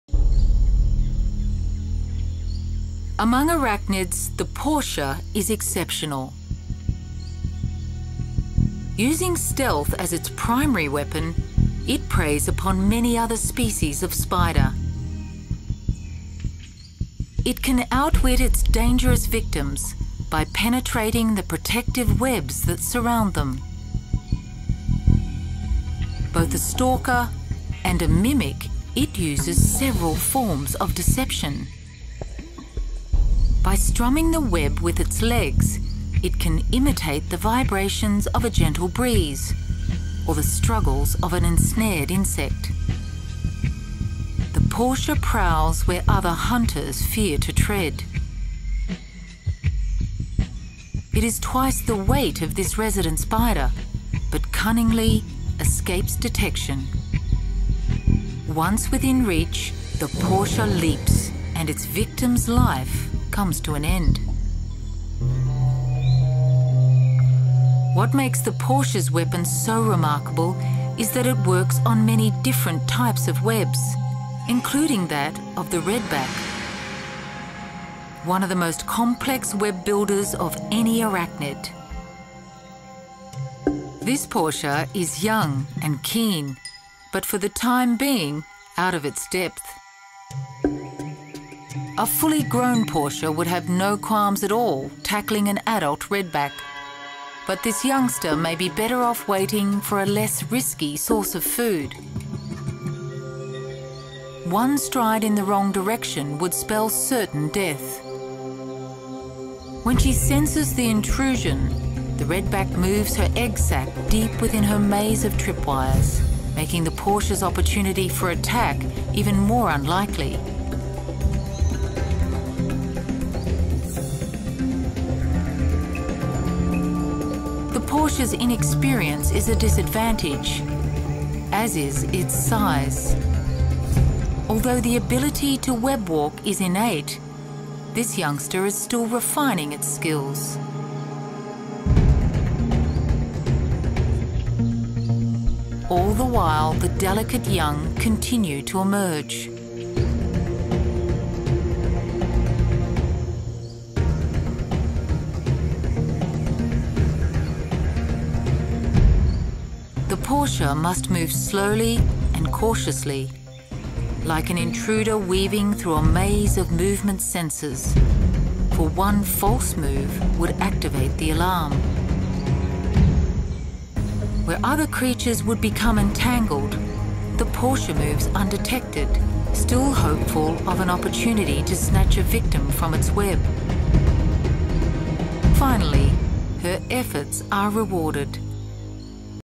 Portia Spider
Portia is a genus of jumping spider which feeds on other spiders (araneophagic). They are remarkable for their intelligent hunting behaviour which suggests they are capable of learning and problem solving, traits normally attributed to much larger animals.